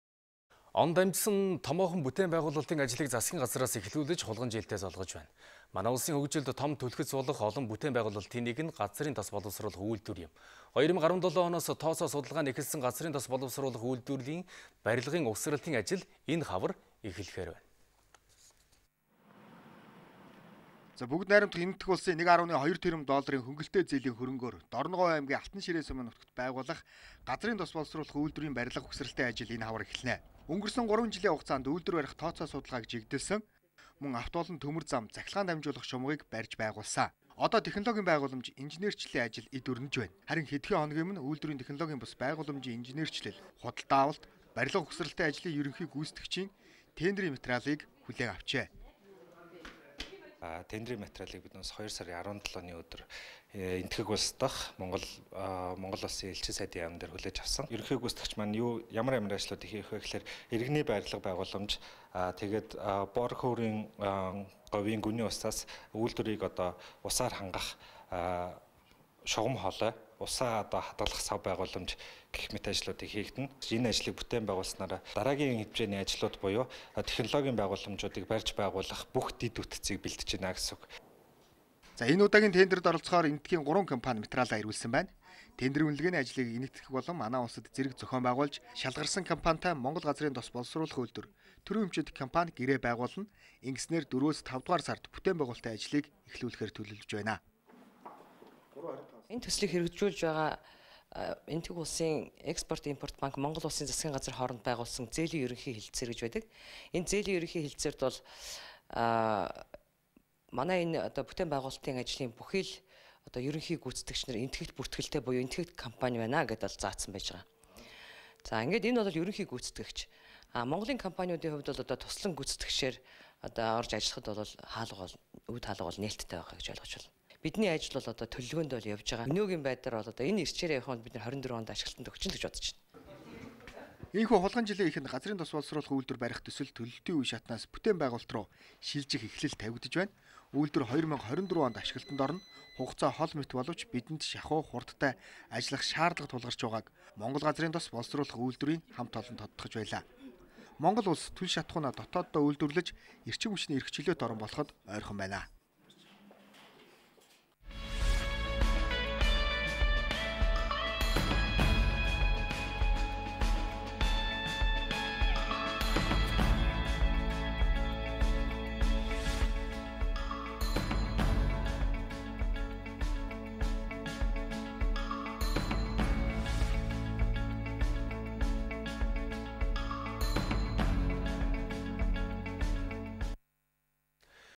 "Газрын тос боловсруулах үйлдвэр"-ийн барилга угсралтын ажил энэ хавар эхэлнэ /2020.02.25/
Он дамжсан томоохон бүтээн байгуулалтын ажлыг Засгийн газраас эхлүүлж хулгана жилтэй золгож байна. Манай улсын хөгжилд том түлхэц болох олон бүтээн байгуулалтын нэг нь "Газрын тос боловсруулах үйлдвэр" юм. 2017 оноос тооцоо судалгаа нь эхэлсэн "Газрын тос боловсруулах үйлдвэр"-ийн барилга угсралтын ажил энэ хавар эхлэхээр байна.